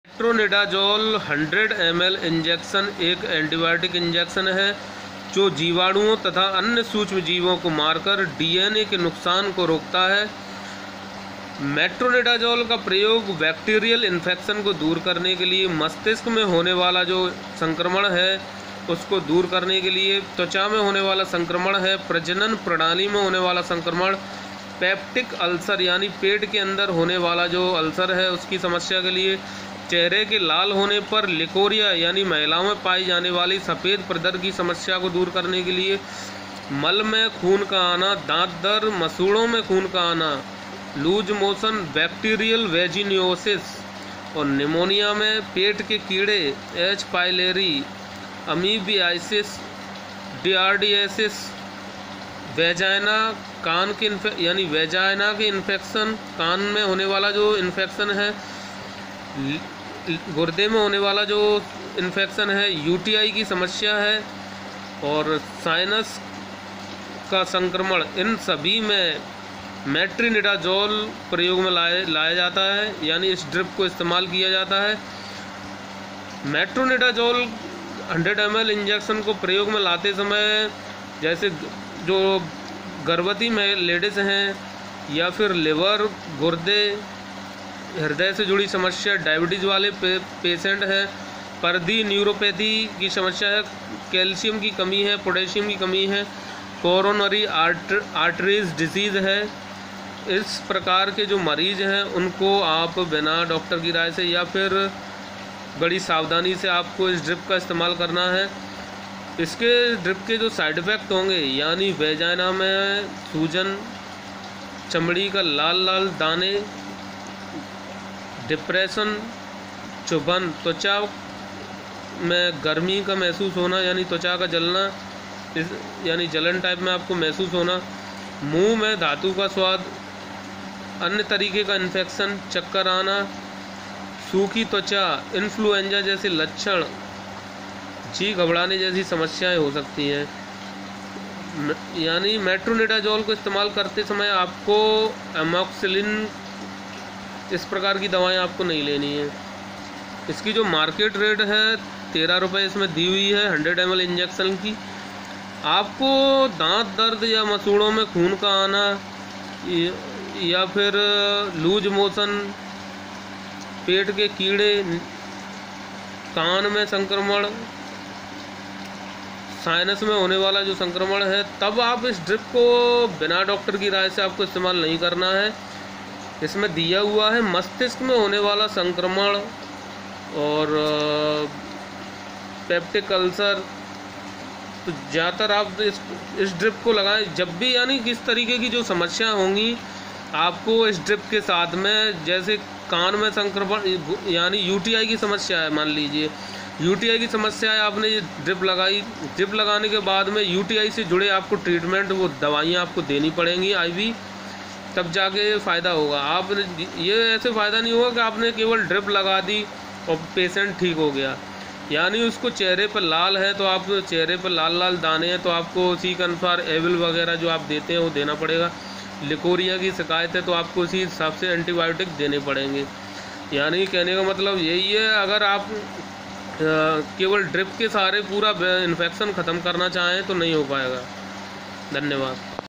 METROGYL / METRO 100ml inj. RX_ Metronidazole Use & side effect
Metrogyl 100 ml injection
     this is antibiotic injection .
 side effects :.  योनि में सूजन , एरिथमा ( चमड़ी पर लाल - 2 दाने ) , depression , त्वचा का जलना , मुँह में धातू का स्वाद  ,संक्रमण , चक्कर आना , शुखी त्वचा , influenza जैसे लक्षण , contact dermatitis , जी घबराना ।
  क्ष